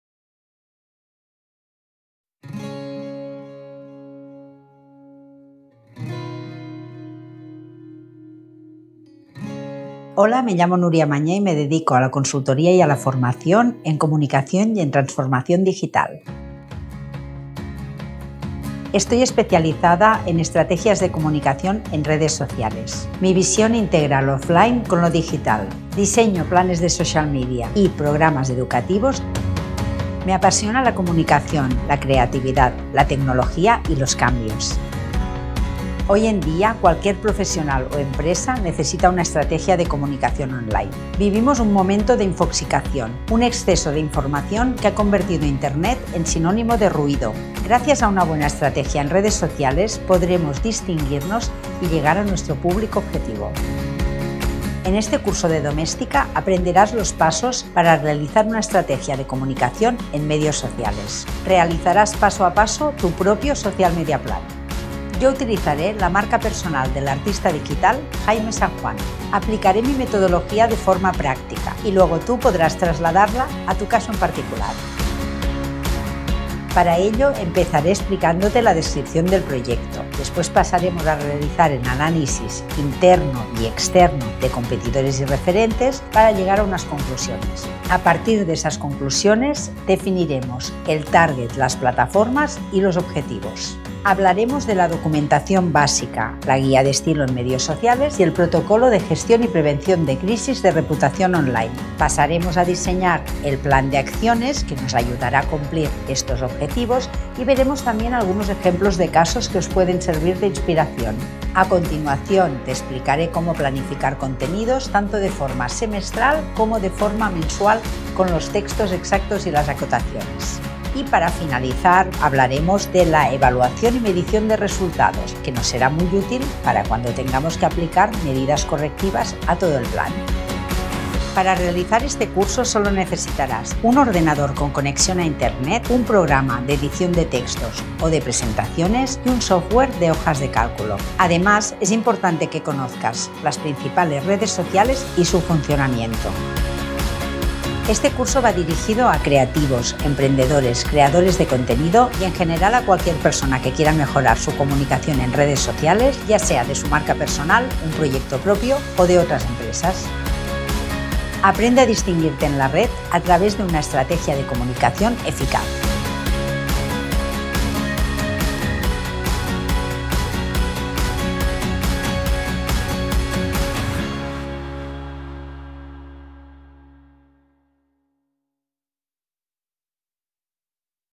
Estrategia de Comunicación para Redes Sociales - Curso online de Núria Mañé | Domestika
Hoy en día cualquier profesional o empresa necesita una estrategia de comunicación online, área en la que Núria Mañé, Consultora de comunicación y Social Media Strategist, ha enfocado su vida profesional desde 2009, desarrollando Social Media Plans listos para ser ejecutados por un Community Manager o cualquier persona para su marca personal.